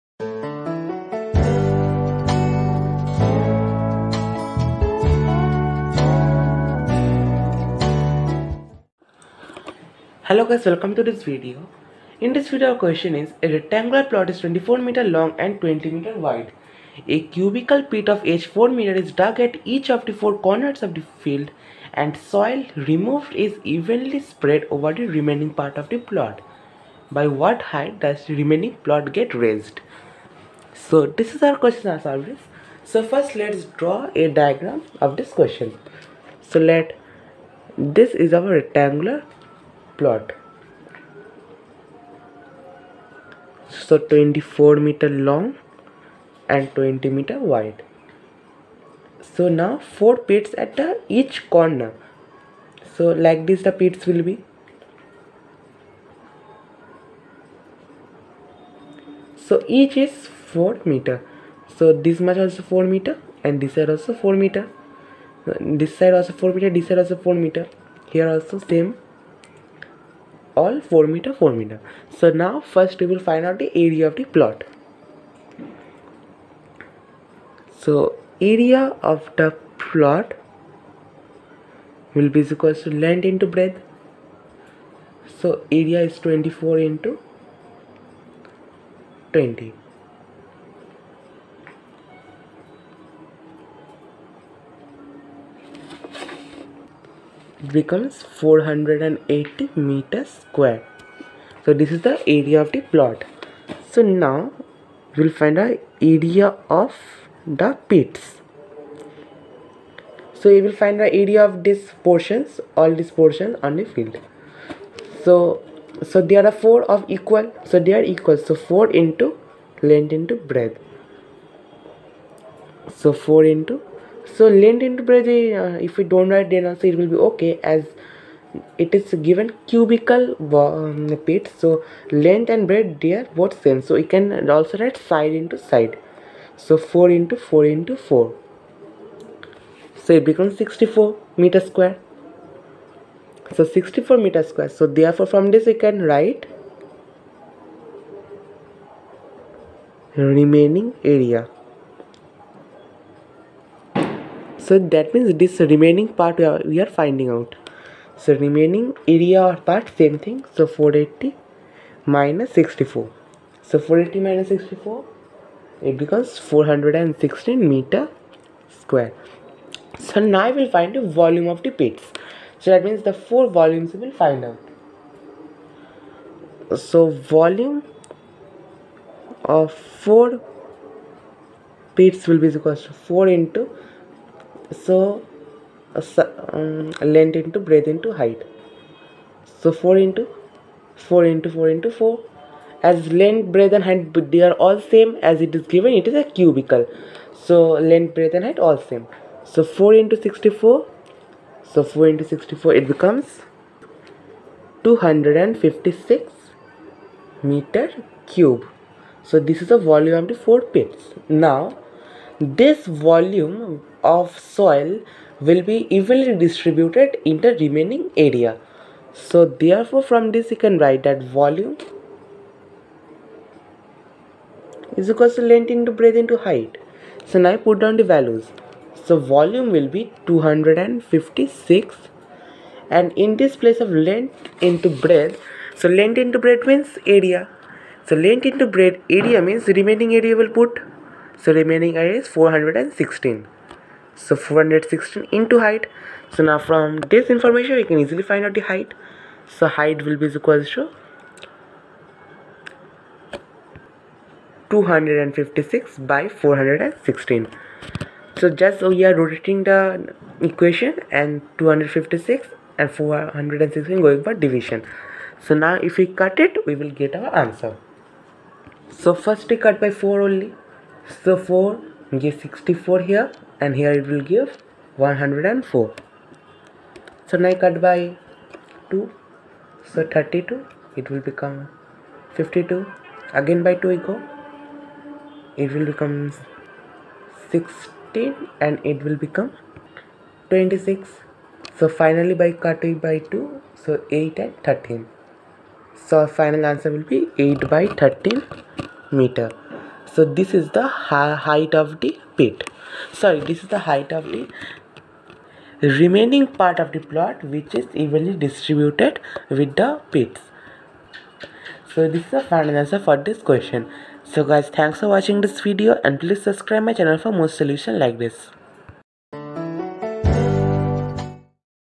A rectangle plot is 24 m long and 20 m wide. A cubical pit of edge 4 m is dug at each of the four…
A rectangle plot is 24 m long and 20 m wide. A cubical pit of edge 4 m is dug at each of the four corners of the field and the soil removed is evenly spread over the remaining part of the plot. By what height does the remaining plot get raised?